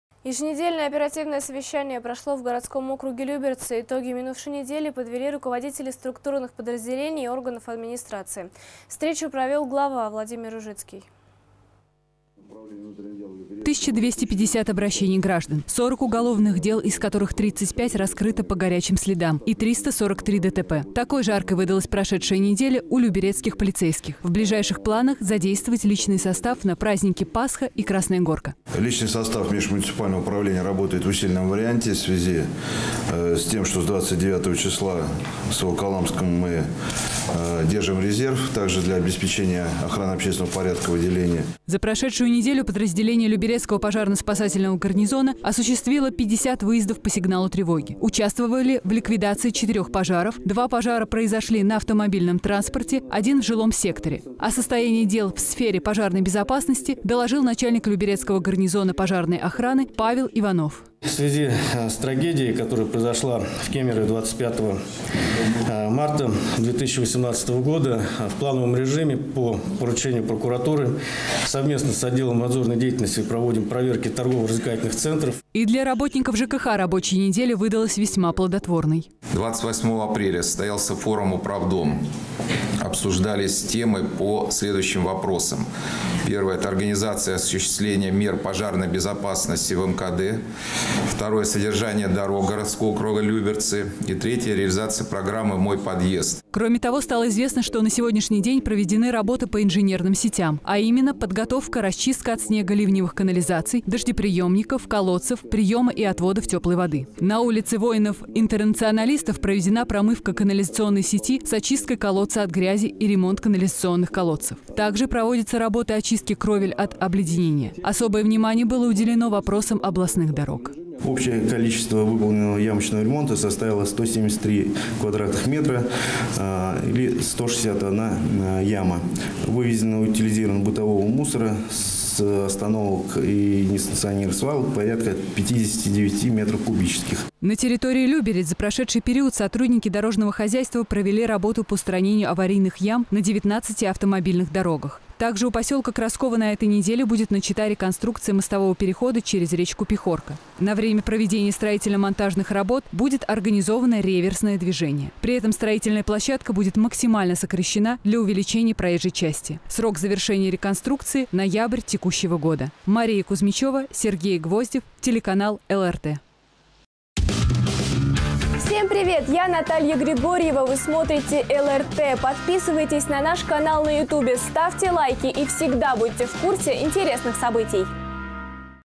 Еженедельное оперативное совещание прошло в администрации
1250 обращений граждан, 40 уголовных дел, из которых 35 раскрыто по «горячим следам» и 343 ДТП. Такой жаркой  выдалась прошедшая неделя  у люберецких полицейских. В ближайших планах задействовать личный состав на праздники Пасха и Красная Горка.
"Личный состав межмуниципального управления работает в усиленном варианте в связи с тем, что с 29 числа с Волоколамском мы держим резерв также для обеспечения охраны общественного порядка".

За прошедшую неделю подразделение Люберецкого пожарно-спасательного гарнизона осуществило 50 выездов по сигналу тревоги. Участвовали в ликвидации 4 пожаров. 2 пожара произошли на автомобильном транспорте, 1 в жилом секторе. О состоянии дел в сфере пожарной безопасности доложил начальник люберецкого гарнизона пожарной охраны, Павел Иванов.
"В  связи с трагедией, которая произошла в Кемерове 25 марта 2018 года в плановом режиме по поручению прокуратуры совместно с отделом надзорной деятельности  проводим  проверки Торгово-развлекательных центров".

И для работников  ЖКХ рабочая неделя выдалась весьма плодотворной.
"28 апреля состоялся форум «Управдом». Обсуждались темы по Следующим вопросам: 1) организация осуществления мер пожарной безопасности в МКД., 2) содержание дорог городского округа Люберцы и 3) реализация программы «Мой подъезд». Присутствующие жители смогли задать и получить на заданные вопросы по ремонту и содержанию многоквартирных домов от представителей управляющих организаций".

Кроме того, стало известно, что на сегодняшний день проведены работы по инженерным сетям. А именно, подготовка, расчистка от снега ливневых канализаций, дождеприемников, колодцев, приема и отводов талой воды. На улице Воинов Интернационалистов проведена  промывка канализационной сети  с очисткой колодца от грязи и ремонт канализационных колодцев. Также  проводятся работы очистки кровель от обледенения.  Особое внимание было уделено  вопросам областных дорог.
"Общее количество выполненного ямочного ремонта составило 173 кв. метра или 161 яма. Вывезено и утилизировано бытового мусора  с остановок и несанкционированных свалок порядка 59 мера кубических".
На территории Люберец за прошедший период сотрудники дорожного хозяйства провели работы по устранению аварийных ям на 19-ти автомобильных дорогах. Также у поселка Красково на этой неделе будет начата реконструкция мостового перехода через речку Пехорка. На время проведения строительно-монтажных работ будет организовано реверсное движение. При этом строительная площадка будет максимально сокращена для увеличения проезжей части. Срок завершения реконструкции –ноябрь текущего года.

Мария Кузмичева, Сергей Гвоздев